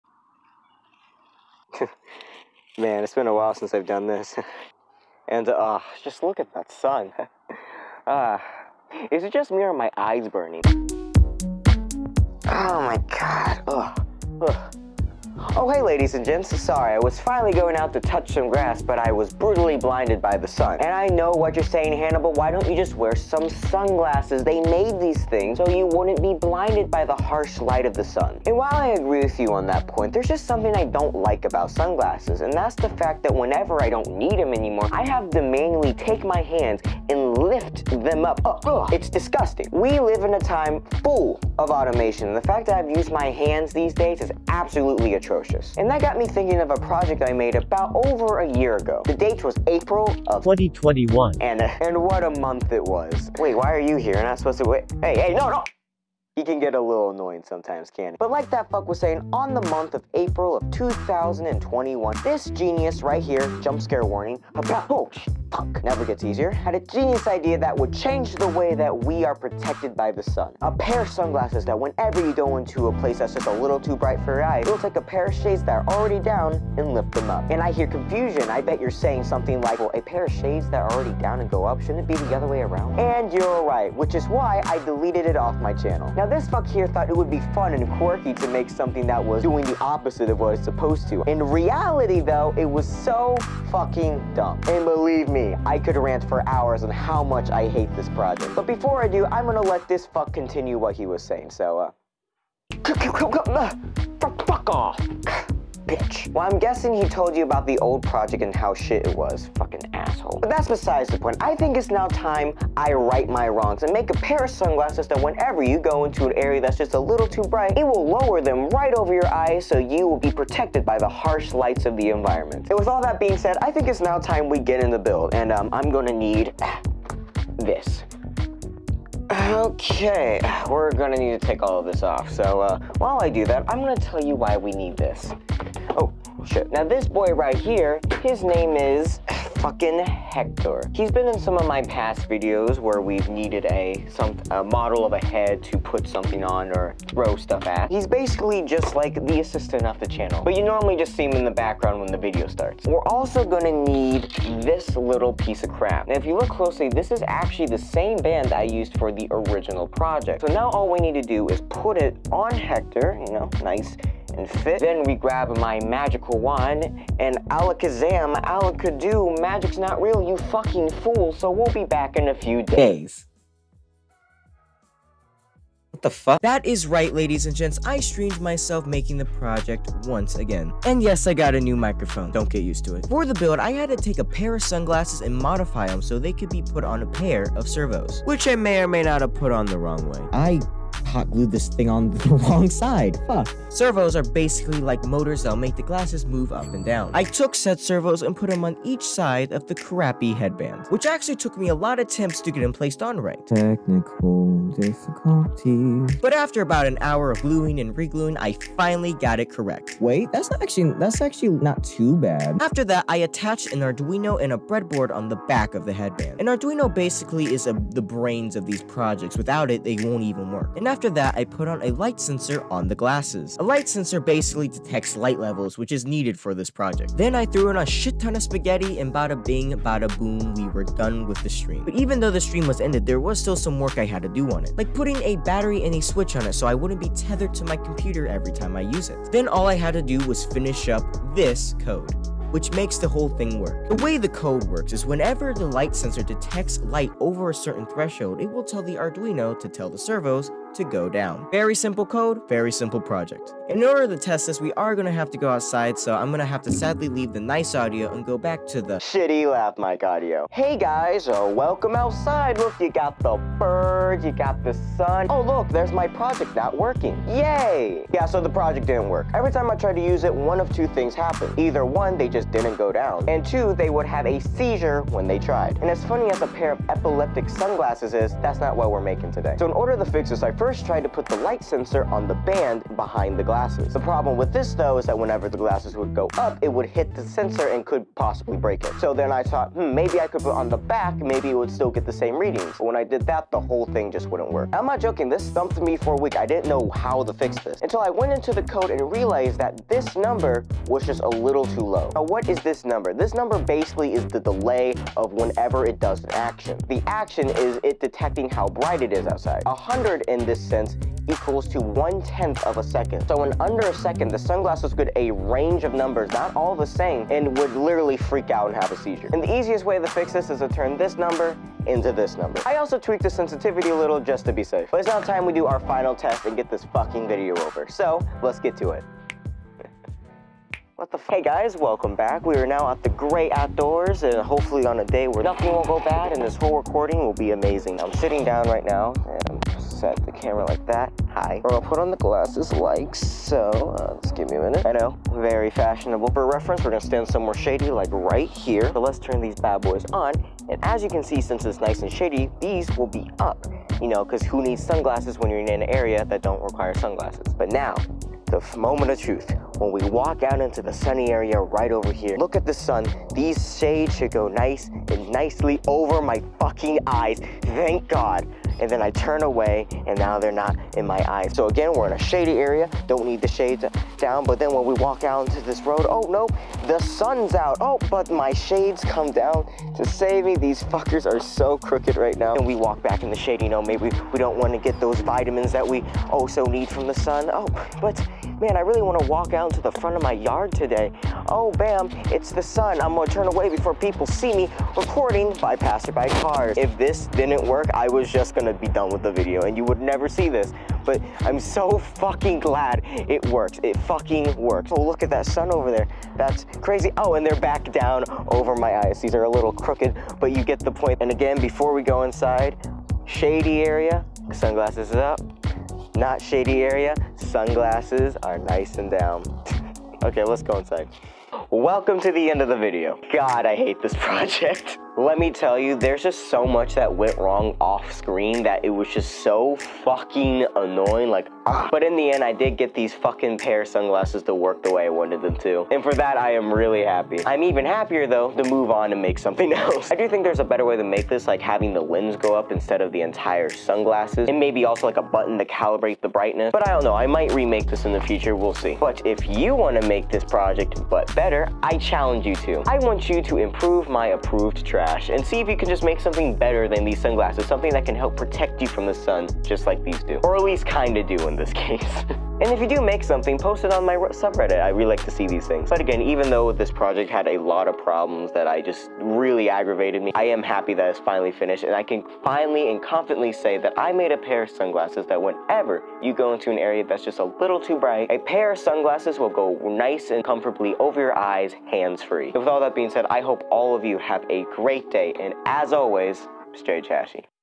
Automatic Sunglasses
I made sunglasses that hate the sun or better put hate you.
HOPE YOU ENOY!!!

0:00 Intro
0:45 Old Project
1:42 Build
4:18 It Didn't Work...
5:31 Final Test
7:01 Outro